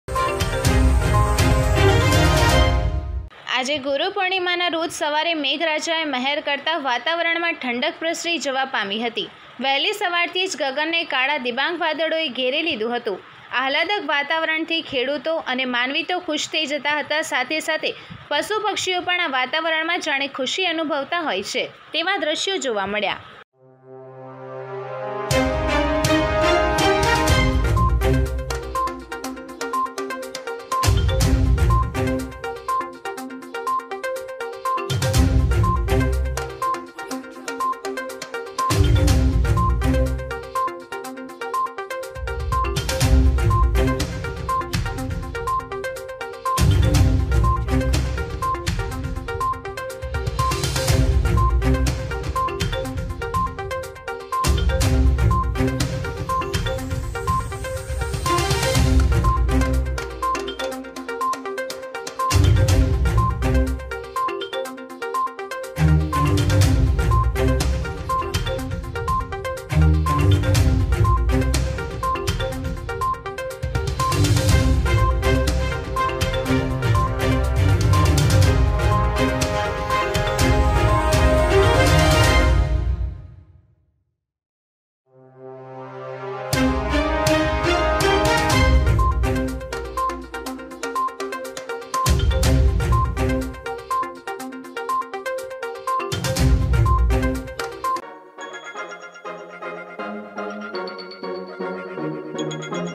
આજે ગુરૂપૂર્ણિમાના રોજ સવારે મેઘરાજાએ મહેર કરતાં વાતાવરણમાં ઠંડક પ્રસરી જવા પામી હતી.
આજે ગુરૂપૂર્ણિમાના રોજ સવારે મેઘરાજાએ મહેર કરતાં વાતાવરણમાં ઠંડક પ્રસરી જવા પામી હતી. વહેલી સવારથી જ ગગનને કાળાં ડીબાંગ વાદળોએ ઘેરી લીધું હતું. આહલાદક વાતાવરણ થી ખેડૂતો, માનવી તો ખુશ થઇ જતા હોય છે સાથે સાથે પશુ પક્ષીઓ પણ આ વાતાવરણમાં જાણે ખુશી અનુભવતા હોય છે.